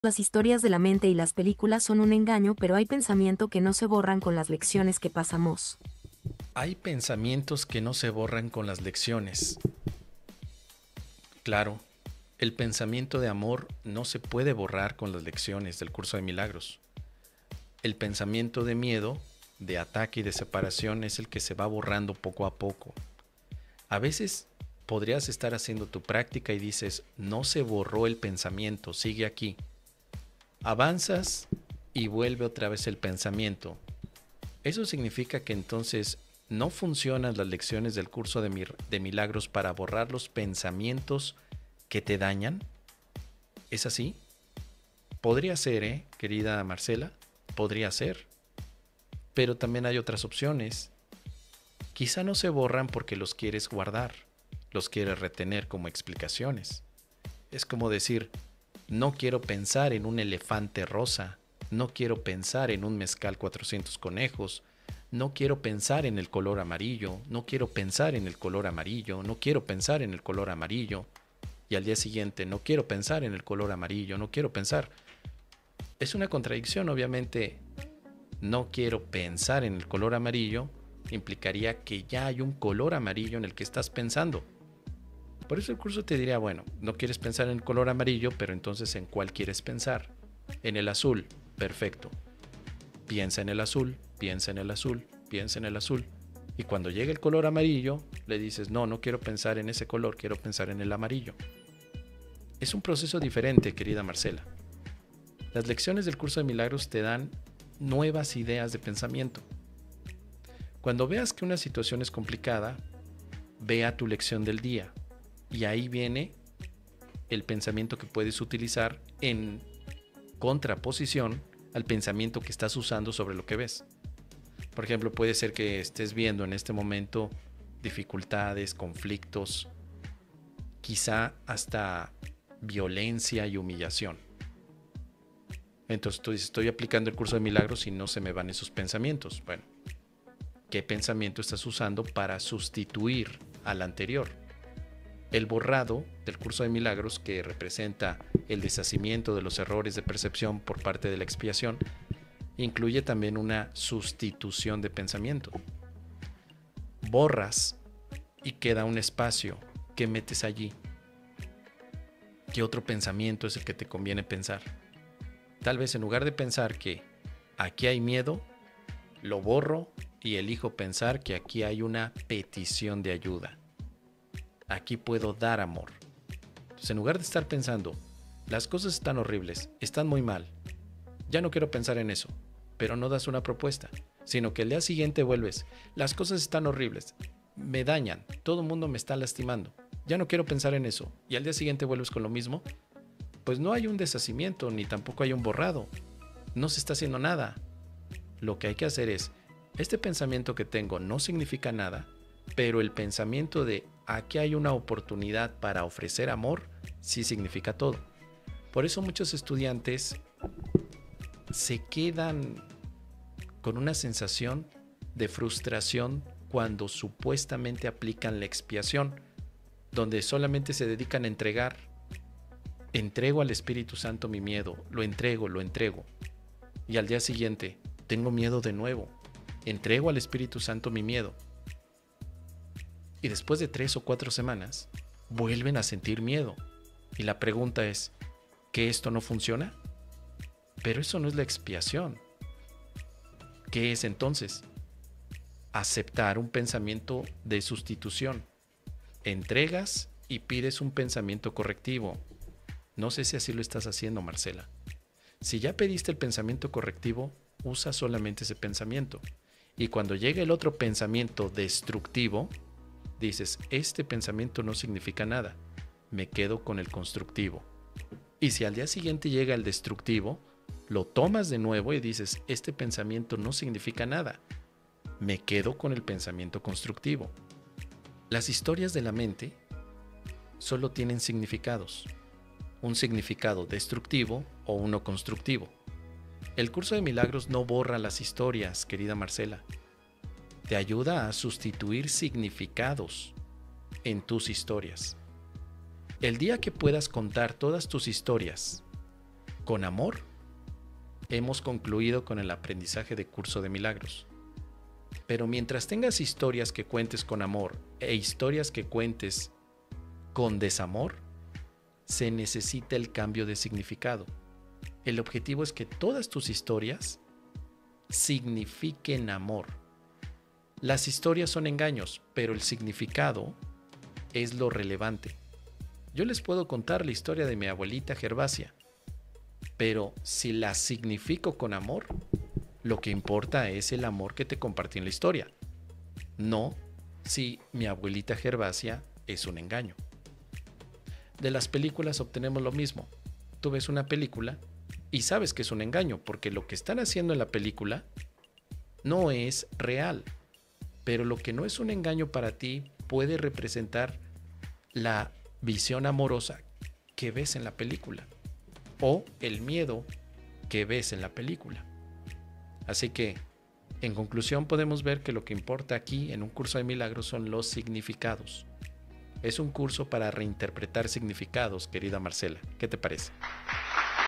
¿Por Qué No Se Borran Algunos Pensamientos? 🧠 Curso de Milagros lo Explica
¿Por qué algunos pensamientos negativos no se borran, incluso al practicar un Curso de Milagros? En esta reflexión, exploramos cómo las lecciones del curso nos ayudan no tanto a "borrar" pensamientos, sino a sustituirlos con ideas de amor y perdón. Descubriremos cómo la expiación nos guía a dejar de lado los pensamientos destructivos y enfocarnos en el amor, cambiando el significado que damos a nuestras historias. ¿Has experimentado dificultades con pensamientos persistentes? ¡Comparte tu experiencia en los comentarios!